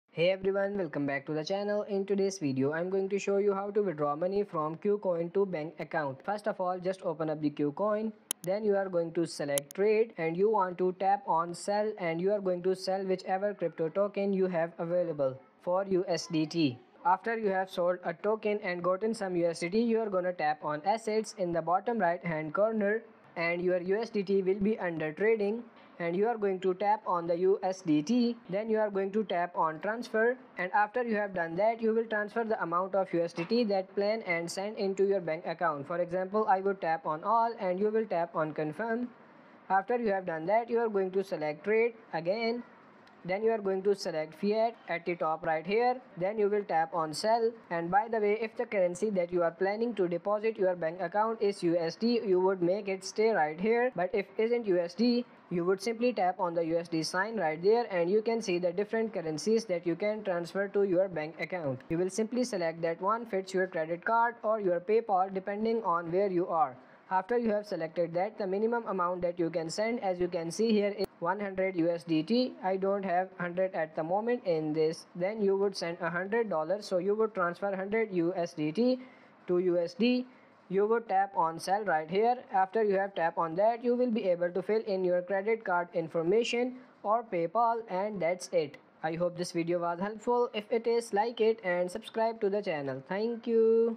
How to Withdraw Money From KuCoin to Bank Account - 2023
If you want to Find How to Withdraw Money From KuCoin to Bank Account but don't know how to do it, then this tutorial is for you!

In this tutorial, I'm going to show you How to Withdraw Money From KuCoin to Bank Account.